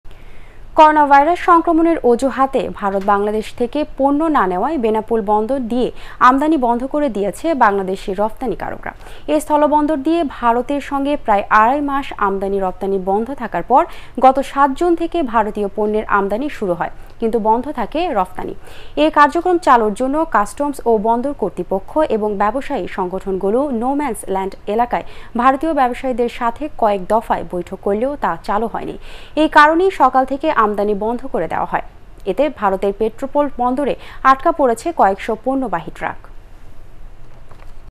ভারত বাংলাদেশ থেকে পণ্য না নেয়ায় বেনাপোল বন্দর দিয়ে আমদানী বন্ধ করে দিয়েছে বাংলাদেশী রফতানীকারকরা।
করোনা ভাইরাস সংক্রমনের অজুহাতে ভারত বাংলাদেশ থেকে পণ্য না নেয়ায় বেনাপোল বন্দর দিয়ে আমদানী বন্ধ করে দিয়েছে বাংলাদেশী রফতানীকারকরা।